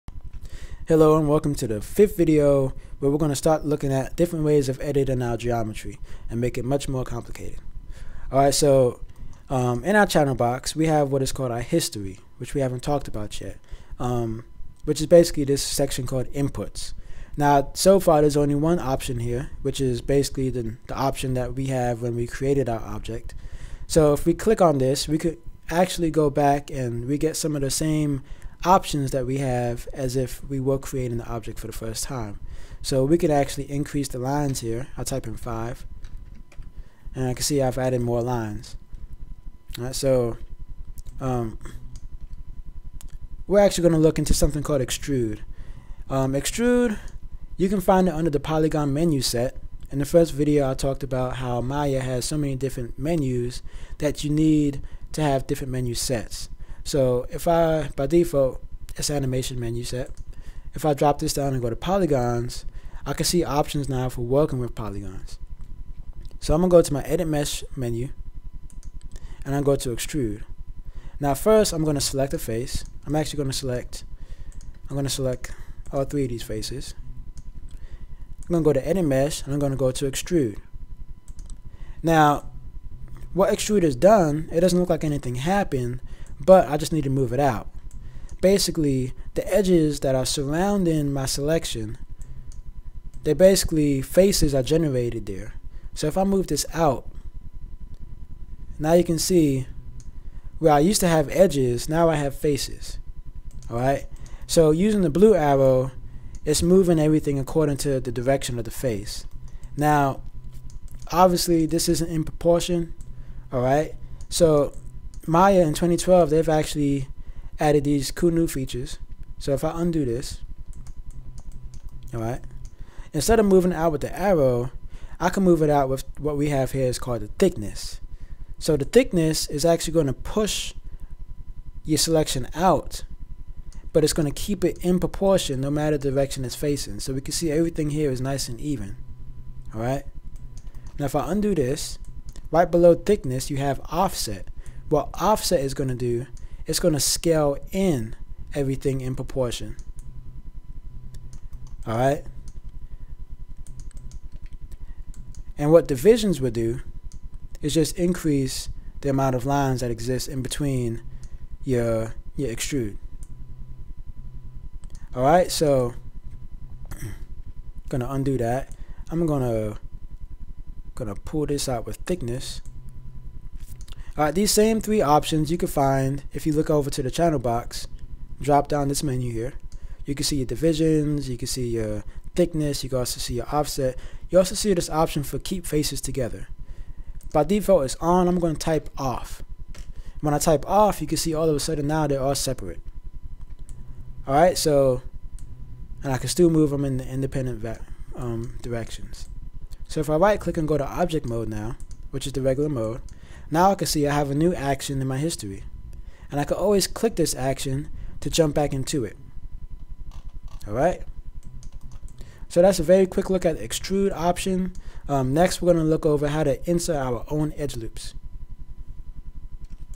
Maya 2012 Tutorial 5 - Extruding
Brief overview of how to extrude polygons in Maya.